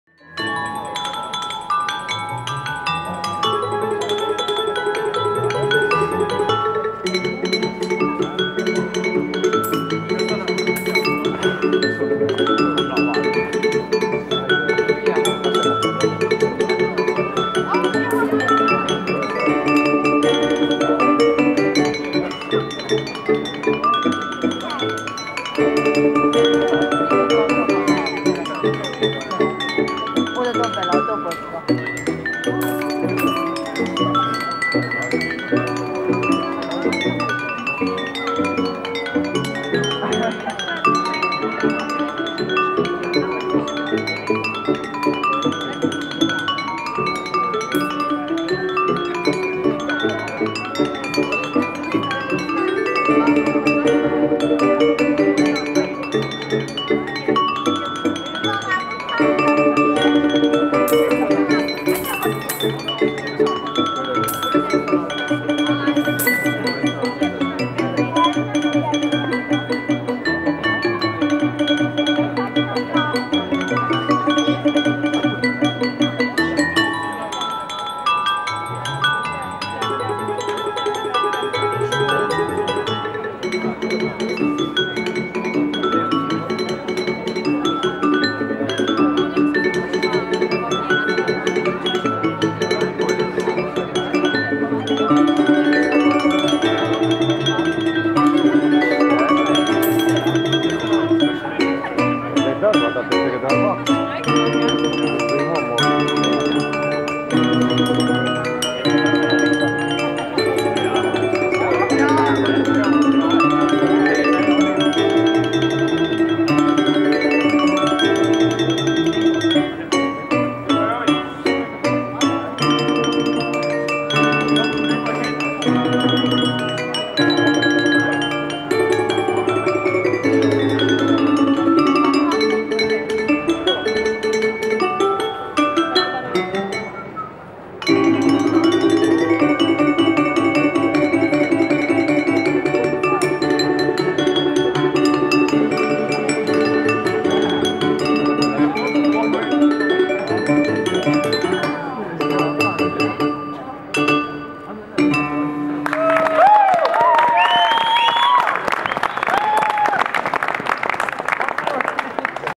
Glass performer in Budapest, Fasion Street
A guy playing on glasses filled with water in Budapest